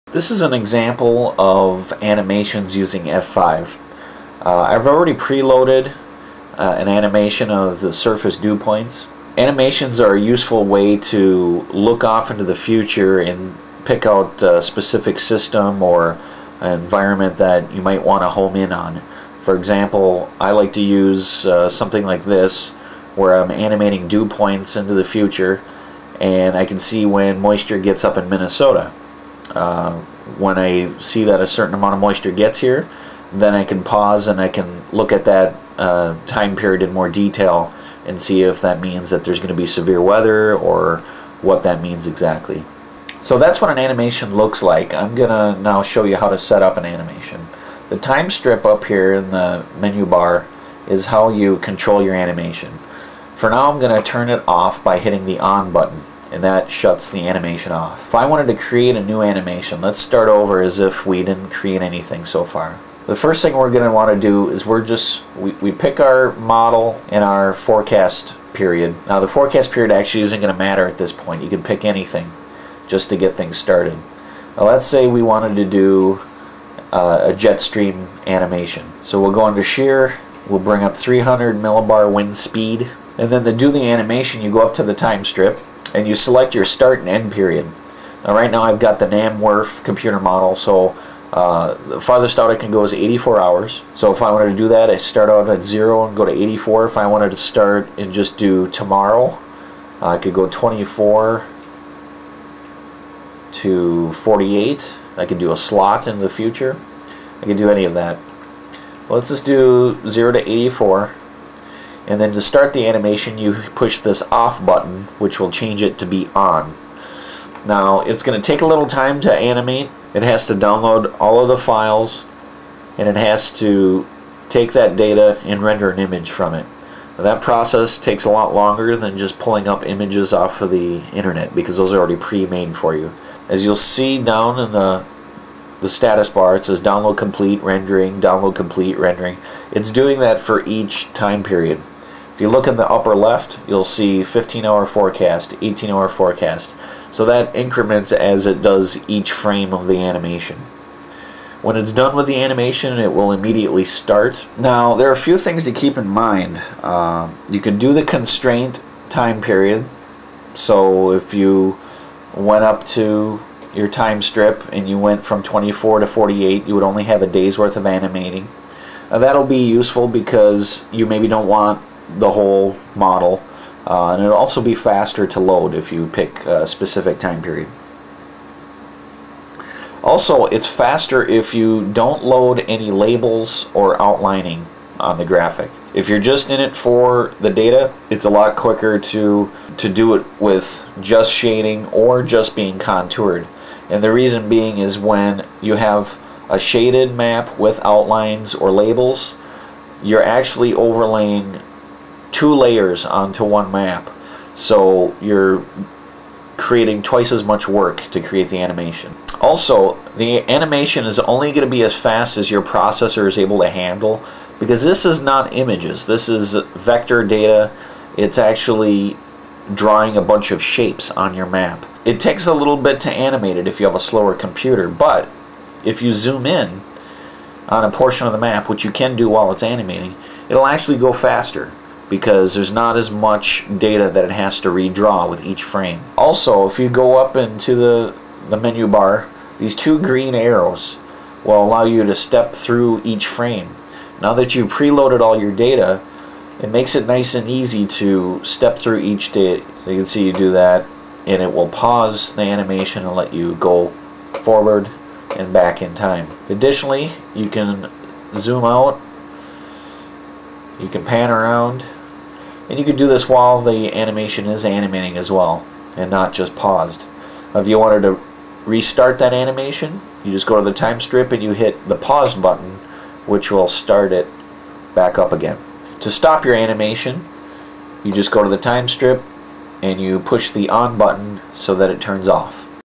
F5Data Weather Forecast Software Tutorial: Animations
This video covers animations, how to optimize them for speed in loading and speed in animating.